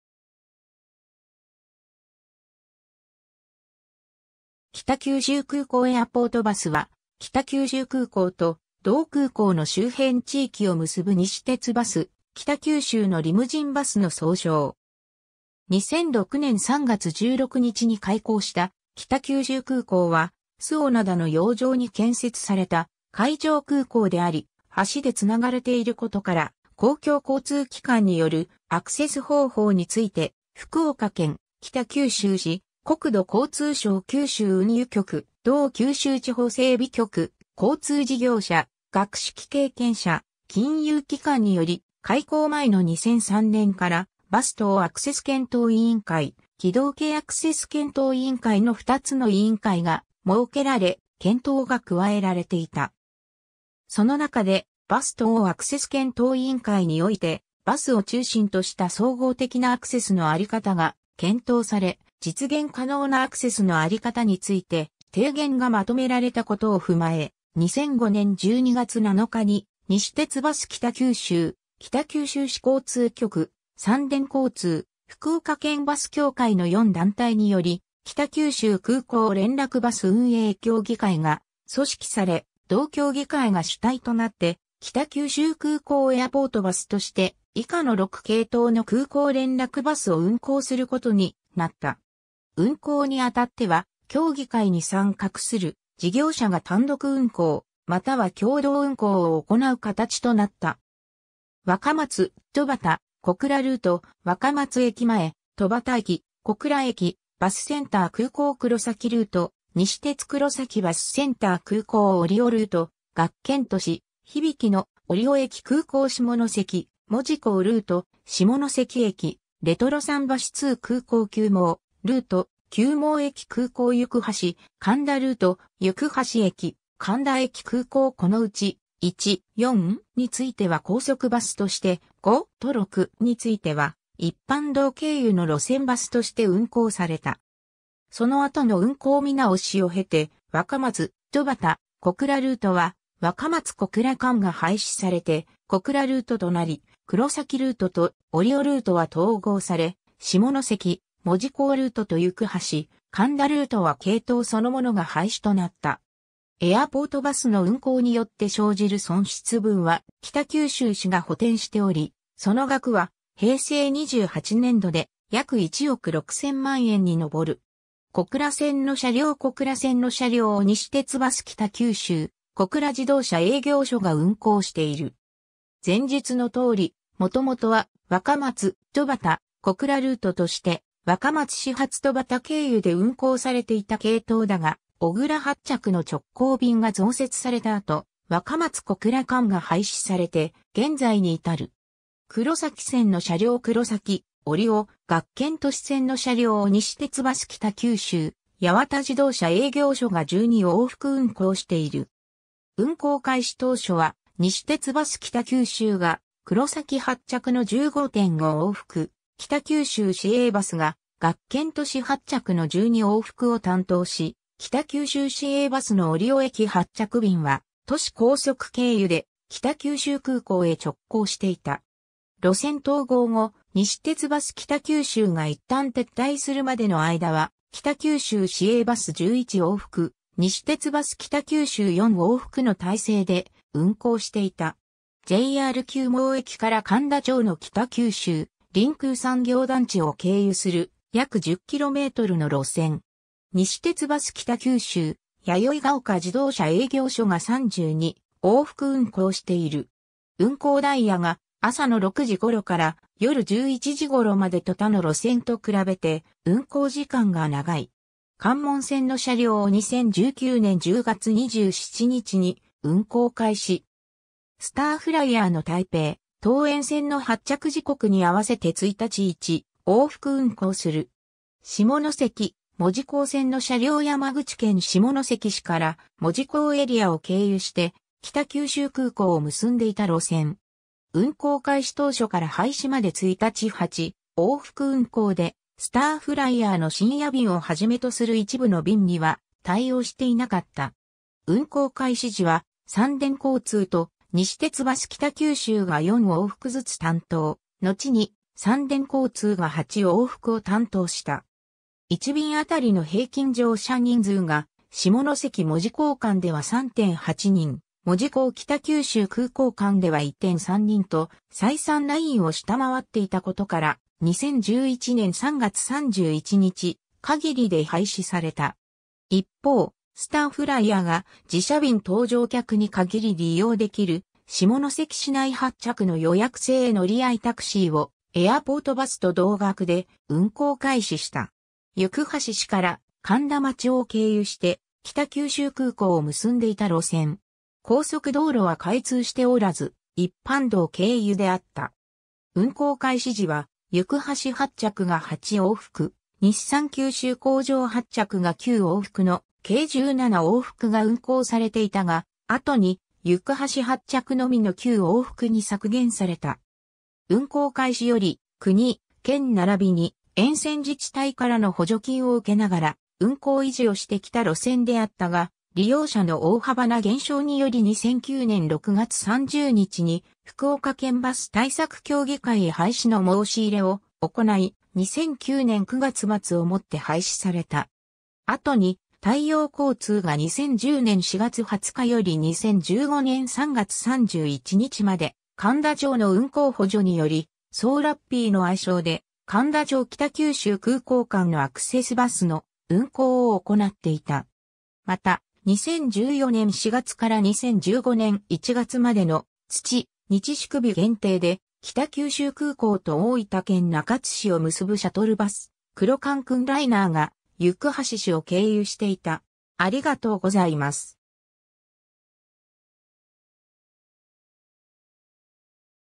北九州空港エアポートバス
北九州空港エアポートバス（きたきゅうしゅうくうこうエアポートバス）は、北九州空港と同空港の周辺地域を結ぶ西鉄バス北九州のリムジンバスの総称。
 2006年3月16日に開港した（新）北九州空港は周防灘の洋上に建設された海上空港であり、橋でつながれていることから、公共交通機関によるアクセス方法について福岡県・北九州市・国土交通省九州運輸局・同九州地方整備局・交通事業者・学識経験者・金融機関により、開港前の2003年から「バス等アクセス検討委員会」「軌道系アクセス検討委員会」の2つの委員会が設けられ、検討が加えられていた。
その中で、「バス等アクセス検討委員会」においてバスを中心とした総合的なアクセスのあり方が検討され、実現可能なアクセスのあり方について「提言」がまとめられたことを踏まえ、2005年12月7日に西鉄バス北九州・北九州市交通局・サンデン交通・福岡県バス協会の4団体により「北九州空港連絡バス運営協議会」が組織され、同協議会が主体となって「北九州空港エアポートバス」として以下の6系統の空港連絡バスを運行することになった。
運行にあたっては、協議会に参画する事業者が単独運行または共同運行を行う形となった。
 若松・戸畑・小倉ルート（西鉄バス北九州運行）：若松駅前・戸畑駅・小倉駅バスセンター - 空港 黒崎ルート（西鉄バス北九州運行）：西鉄黒崎バスセンター - 空港 折尾ルート（北九州市交通局運行）：学研都市ひびきの・折尾駅 - 空港 下関・門司港ルート（西鉄バス北九州・サンデン交通共同運行）：下関駅・レトロ桟橋通 - 空港 朽網ルート（西鉄バス北九州運行）：朽網駅 - 空港 行橋・苅田ルート（西鉄バス北九州運行）：行橋駅・苅田駅 - 空港 このうち、1. - 4.については高速バスとして、5.と6.については一般道経由の路線バスとして運行された。
 その後の運行見直しを経て、若松・戸畑・小倉ルートは若松 - 小倉間が廃止されて小倉ルートとなり、黒崎ルートと折尾ルートは統合され、下関・門司港ルートと行橋・苅田ルートは系統そのものが廃止となった。
 エアポートバスの運行によって生じる損失分は北九州市が補填しており、その額は平成28年度で約1億6千万円にのぼる。
 小倉線の車両（2008年） 小倉線の車両（2017年） 西鉄バス北九州・小倉自動車営業所（北九州市小倉北区）が運行している。
前述のとおり、元々は「若松・戸畑・小倉ルート」として若松始発戸畑経由で運行されていた系統だが、小倉発着の直行便（ノンストップ便）が増設された後、若松 - 小倉間が廃止されて現在に至る。
 黒崎線の車両（2006年） 黒崎・折尾・学研都市線の車両（2016年） 西鉄バス北九州・八幡自動車営業所（北九州市八幡西区）が12往復運行している（このうち5往復は西鉄黒崎バスセンター発着）。
 運行開始当初は西鉄バス北九州が黒崎発着の15.5往復、北九州市営バスが学研都市発着の12往復を担当し、北九州市営バスの折尾駅発着便は都市高速経由で北九州空港へ直行していた。
路線統合後、西鉄バス北九州が一旦撤退するまでの間は、北九州市営バス11往復・西鉄バス北九州4往復の体制で運行していた。
 JR朽網駅から苅田町の北九州臨空産業団地を経由する約10kmの路線。
西鉄バス北九州・弥生が丘自動車営業所（北九州市小倉南区）が32往復運行している。
運行ダイヤが朝の6時ごろから夜11時ごろまでと他の路線と比べて運行時間が長い。
 関門線の車両（2019年） 2019年10月27日に運行開始。
スターフライヤーの台北/桃園線の発着時刻に合わせて1日1往復運行する。
 下関・門司港線の車両（2008年） 山口県下関市から門司港エリアを経由して北九州空港を結んでいた路線。
運行開始当初から廃止まで1日8往復運行で、スターフライヤーの深夜便をはじめとする一部の便には対応していなかった。
運行開始時はサンデン交通（東駅営業所）と西鉄バス北九州（門司自動車営業所）が4往復ずつ担当、のちにサンデン交通が8往復を担当した。
 1便あたりの平均乗車人数が下関 - 門司港間では3.8人、門司港 - 北九州空港間では1.3人と採算ラインを下回っていたことから、2011年3月31日限りで廃止された。
一方、スターフライヤーが自社便搭乗客に限り利用できる下関市内発着の予約制乗合タクシー（下関山電タクシー運行）をエアポートバスと同額で運行開始した。...